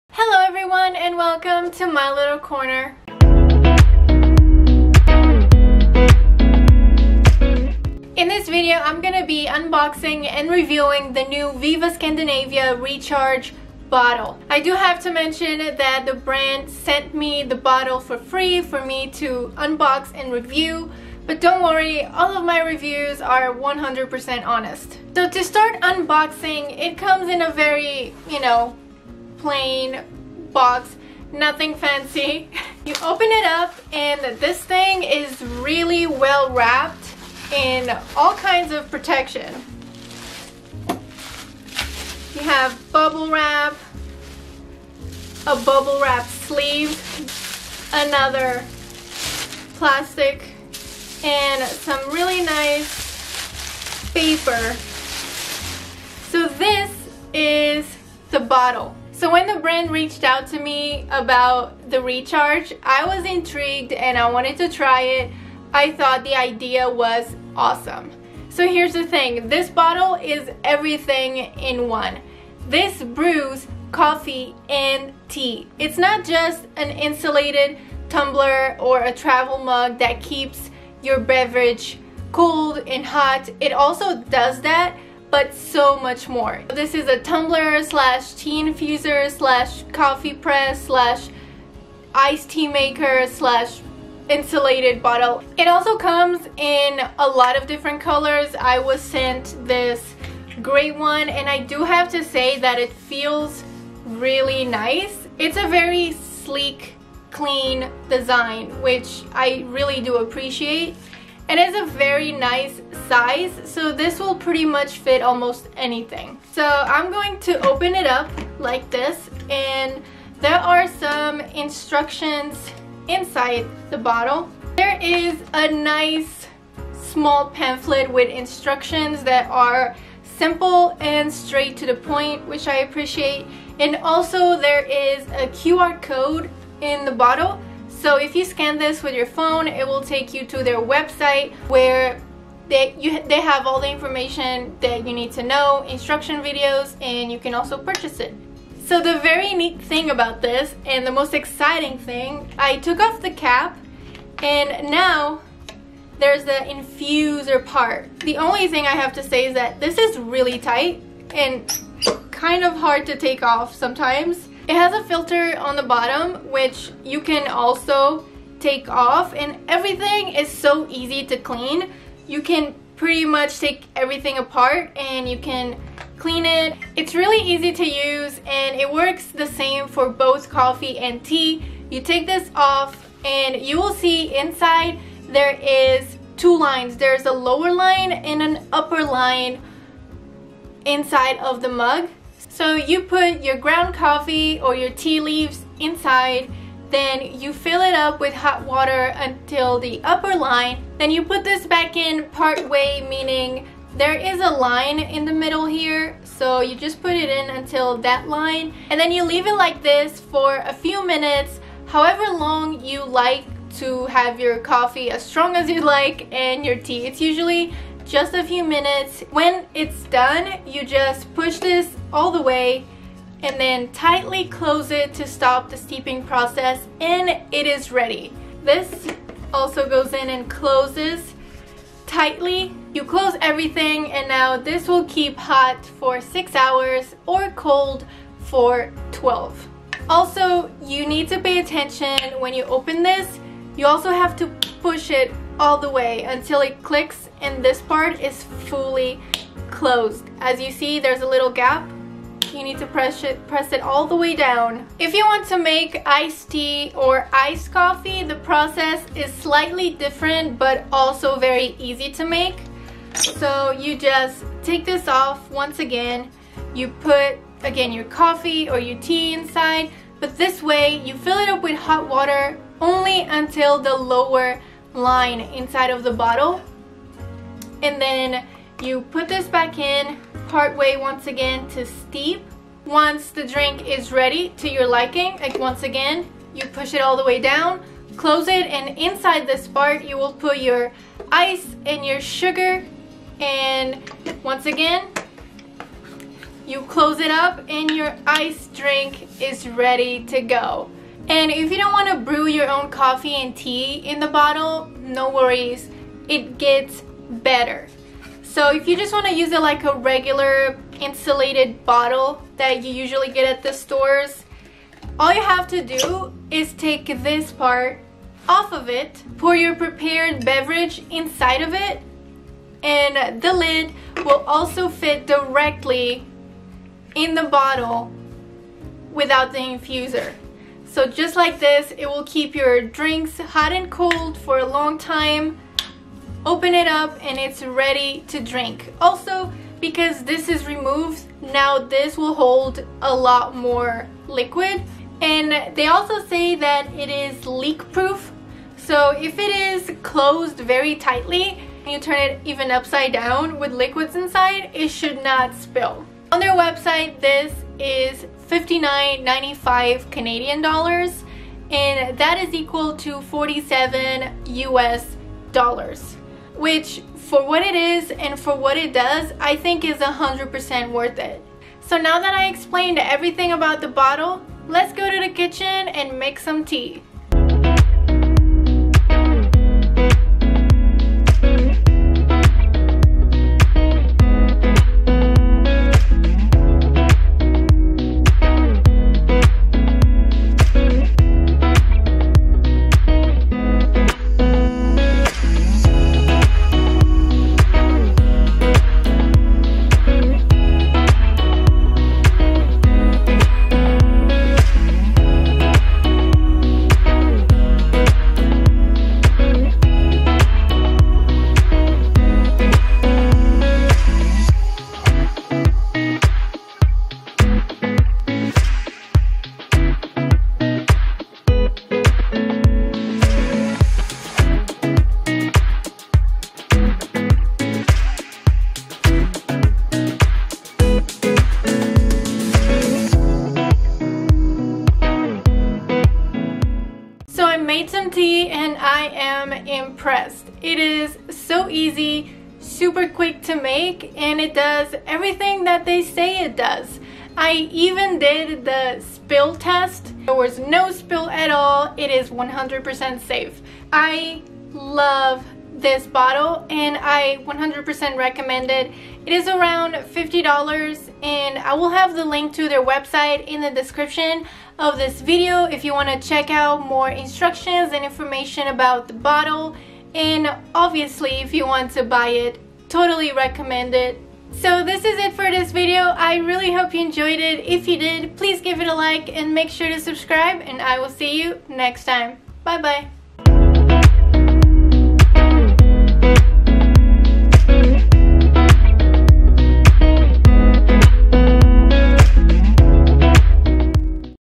The Only Travel Mug You'll Ever Need! | VIVA Scandinavia Recharge | Review
Welcome to my Little Corner!
In this video I unbox and review the all-in-one VIVA Scandinavia Recharge travel mug!

Camera: Canon EOS M50
Lights: Fovitec Studio Pro Softboxes
Microphone: Movo VXR10